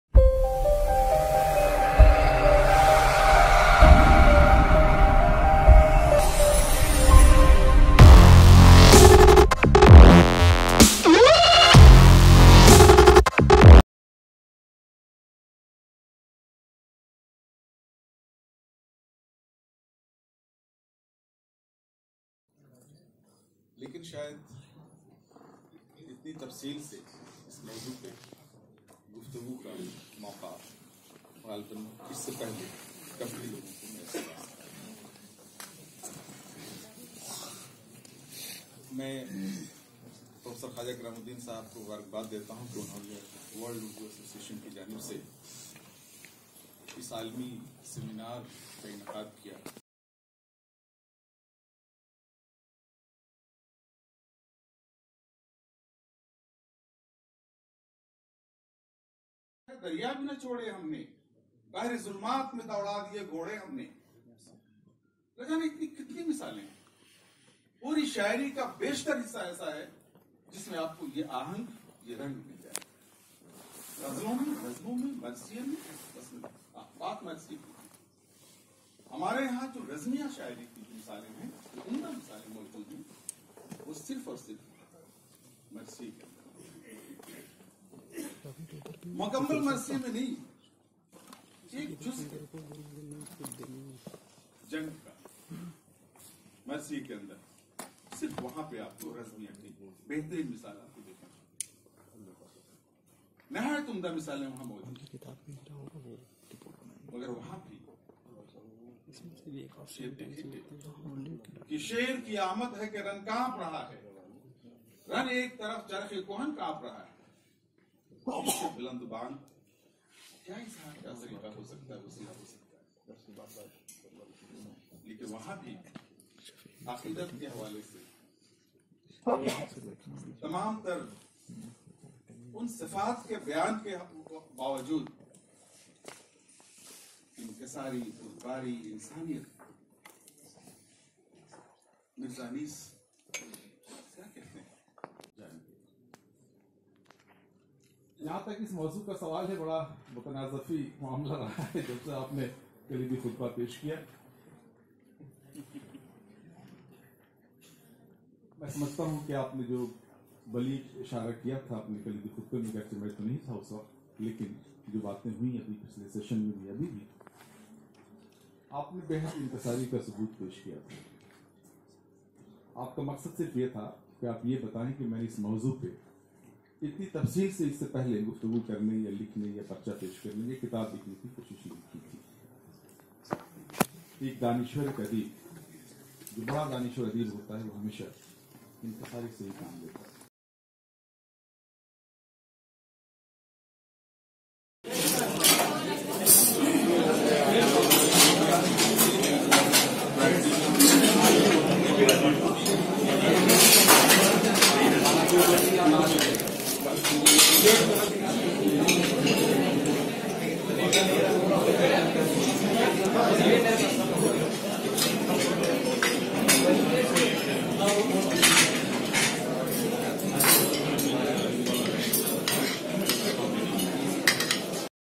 Prof. Anwar Alam Pasha presides International Urdu Seminar (JNU. New Delhi)پروفیسر انوار عالم پاشا۔
Prof. Anwar Alam Pasha, the prominent Urdu critic, poet, and political mentor presiding over the International Urdu Seminar on “Epic Poetry in Urdu, Persian & Arabic: A Saga of War and Peace” at Jawaharlal Nehru University (New Delhi). The function was
organized by Urdu Department and World Urdu Association. Prof Pasha is the former chairperson of Urdu department of JNU and currently a professor in the same department. He also enjoys the position of the chairperson in the Governing Body of Acharya Narendra Dev College, Delhi University.

shukriya